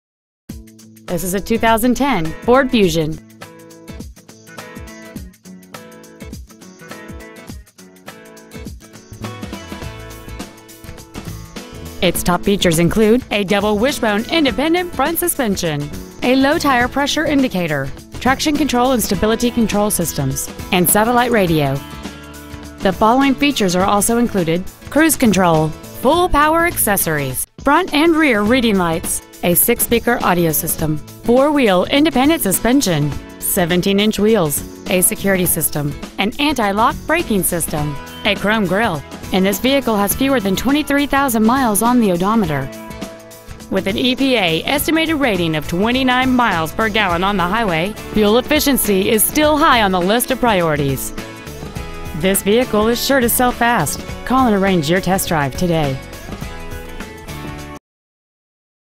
2010 Ford Fusion Sheffield Village OH
Year : 2010
 Make : Ford
 Model : Fusion
 Engine : 2.5L I4 16V MPFI DOHC

 Exterior : GRAY
 Miles : 22,069
 Interior : BLACK

 Pre-Owned 2010 Ford Fusion Sheffield Village  OH